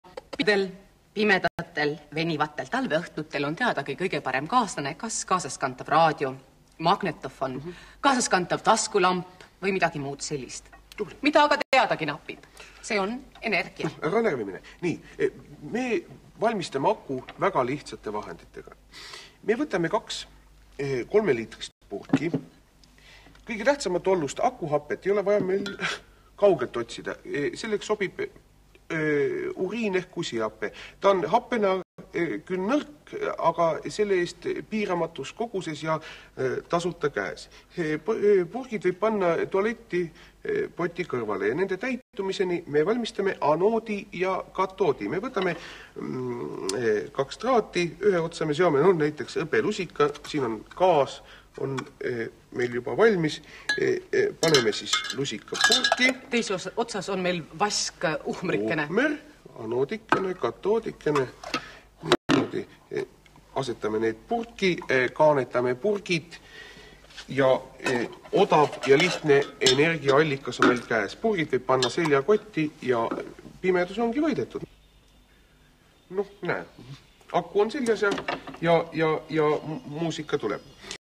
Teeme ise aku - akupanga
Isetehtud mobiilne ja kaasaskantav aku. Teeme seda ise - OMA KODUS!